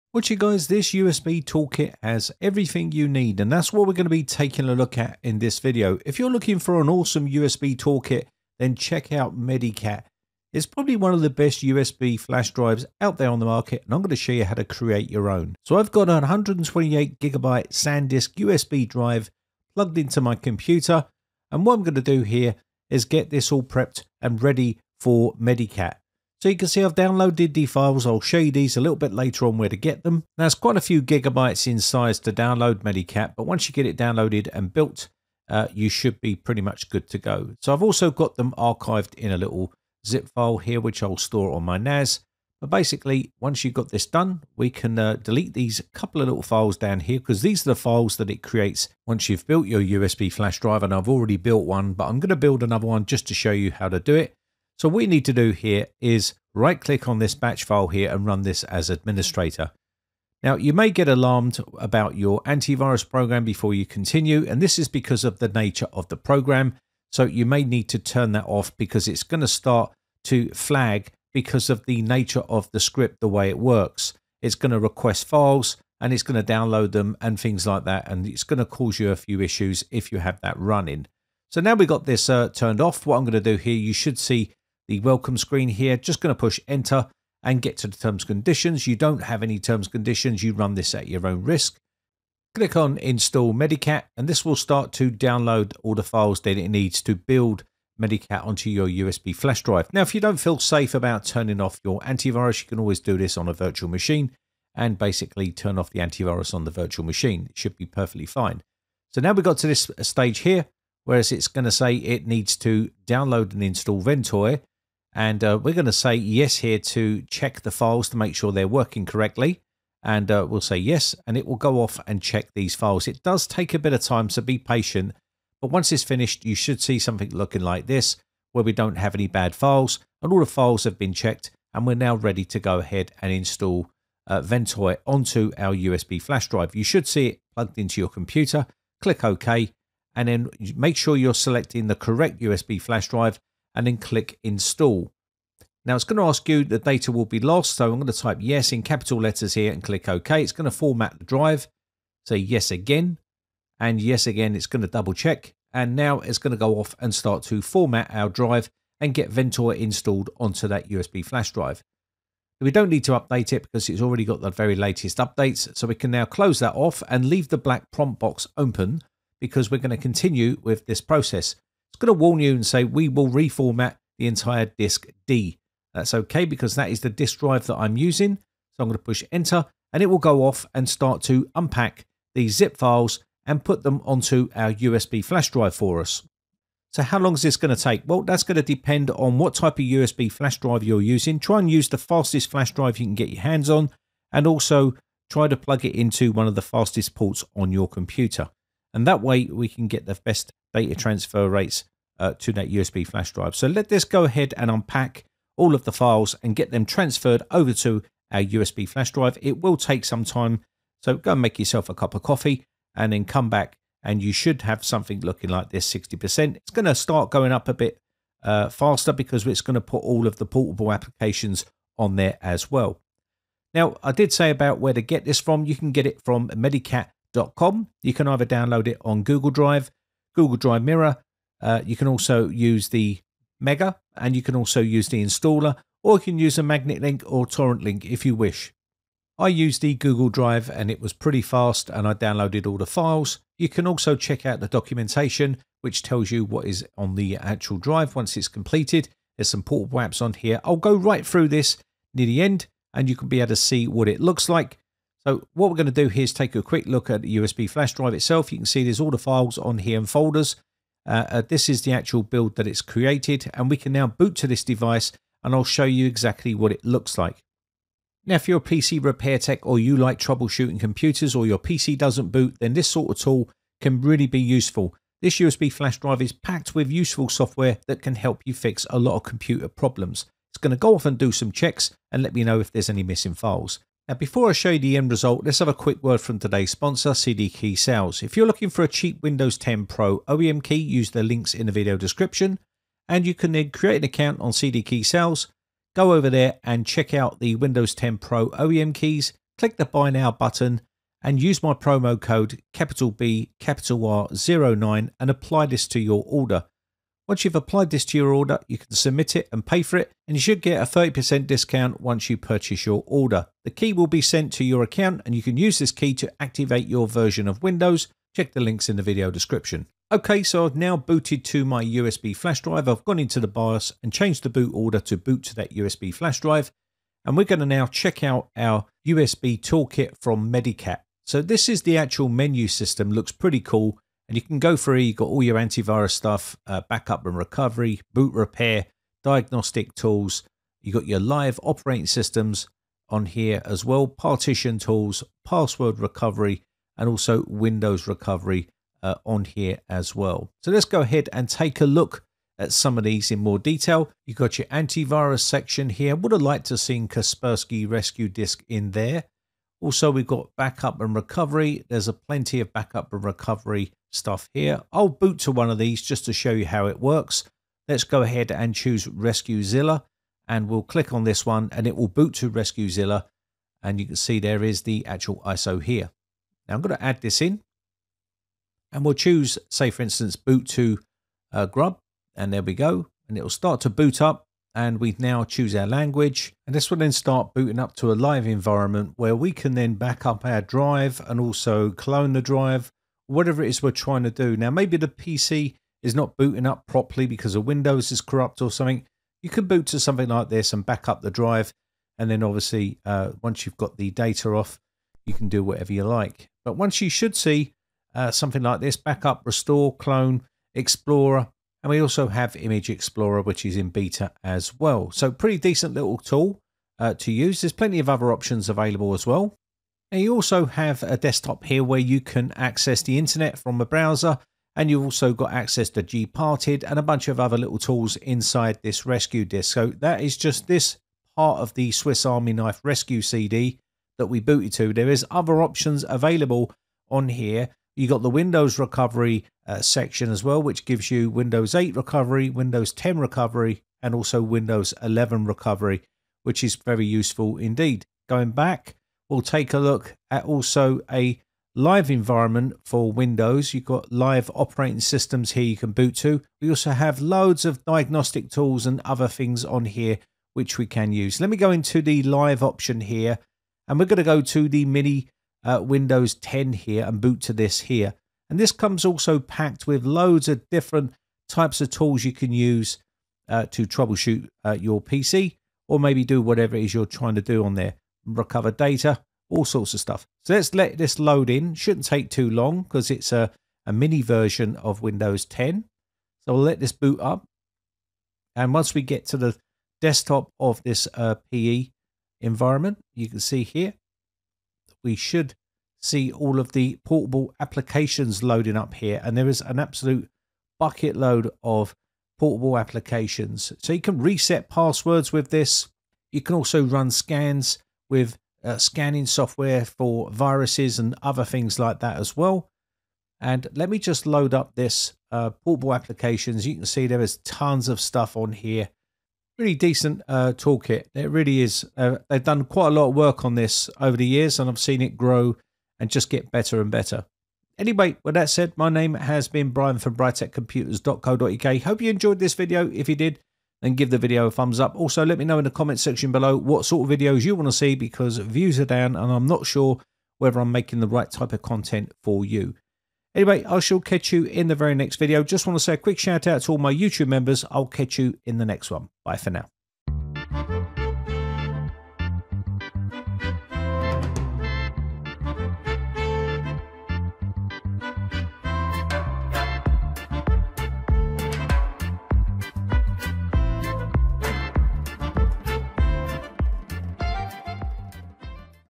This USB Toolkit Has Everything You Need
So you want to create the ultimate USB toolkit and don't know how? well I will show you only USB toolkit you will ever need, its called Medicat.
This is probably the ultimate USB toolkit for pc repair techs.